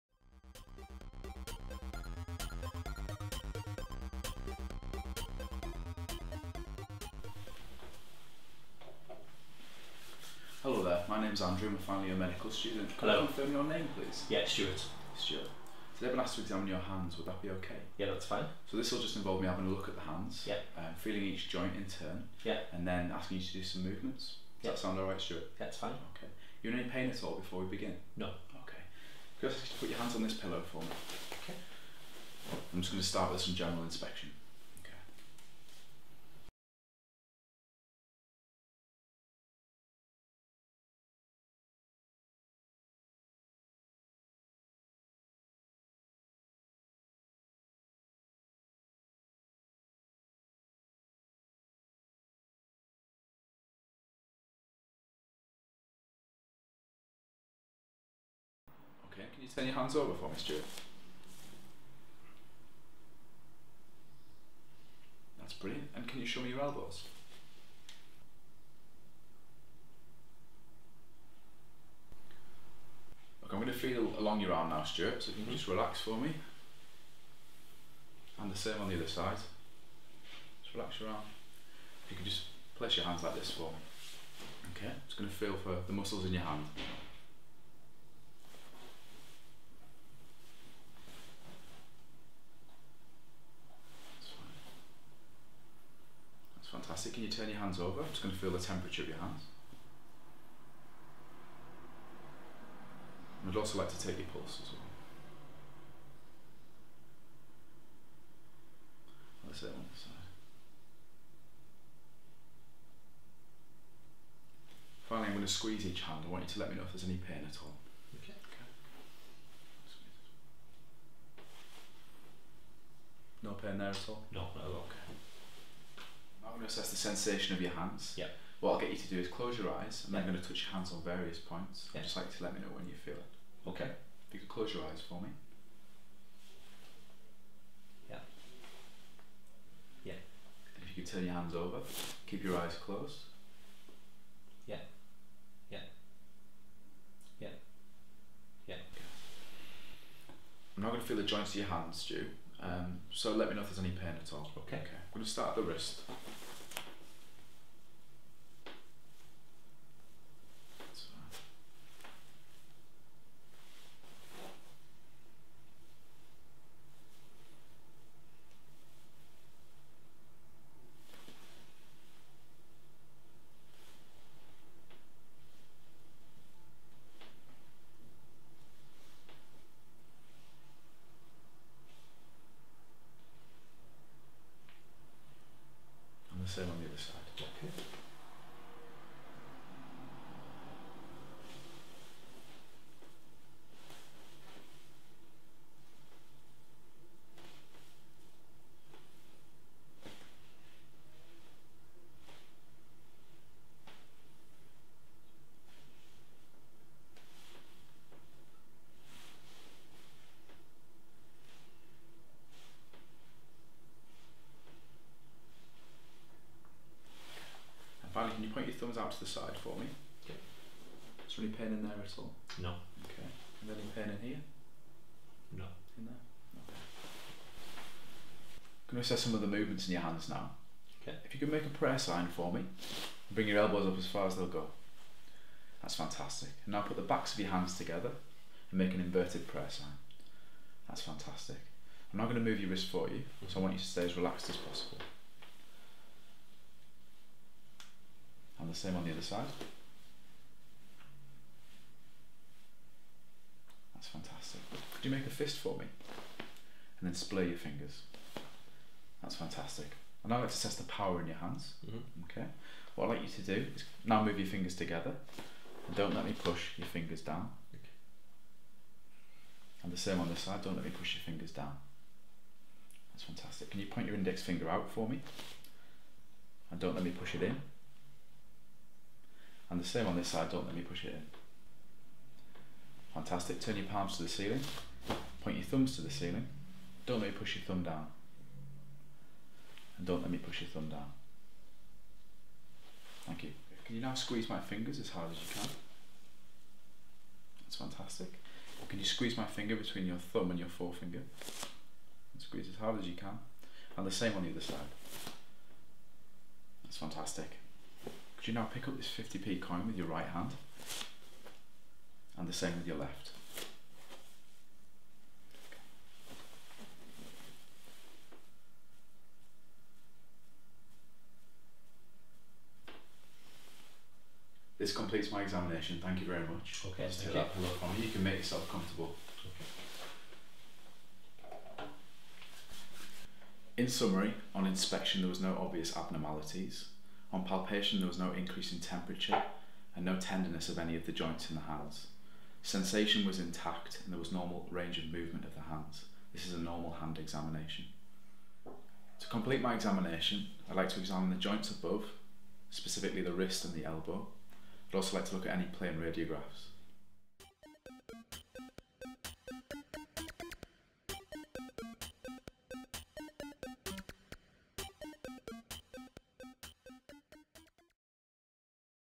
Hand Examination - OSCE Guide (old version) | UKMLA | CPSA | PLAB 2
Chapters:
- Introduction 00:00
- General inspection 00:33
- Palpate wrist 01:12
- Sensation 02:07
- Palpate hand joints 02:32
- Movement 03:51
- Power 04:20
- Hand function 04:55
- Tinel’s test 05:18
- Summarise 05:32